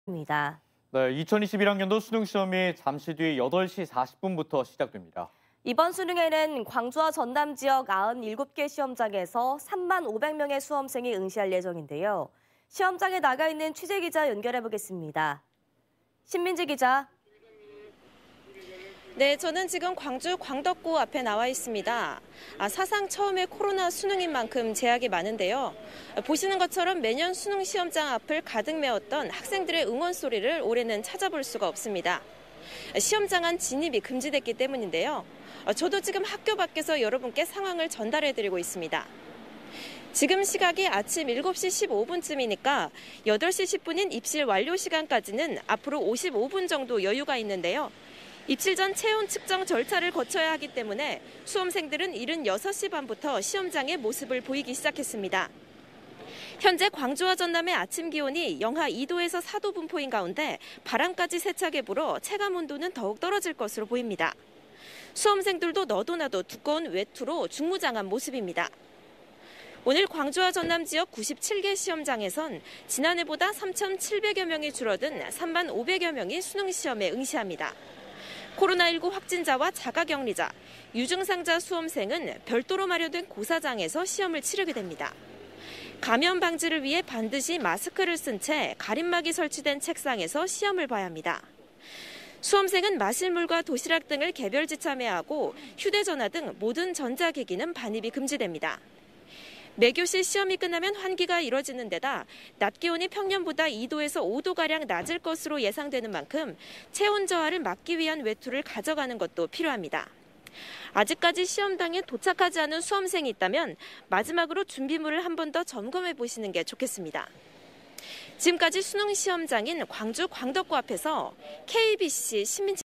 오늘 수능..한파 속 수험생 속속 입실
2021학년도 수능 시험이 잠시 뒤 8시 40분부터 시작됩니다.

이번 수능에는 광주와 전남지역 97개 시험장에서 3만 5백 명의 수험생이 응시할 예정인데요.

시험장에 나가있는 취재기자 연결하겠습니다. 신민지 기자!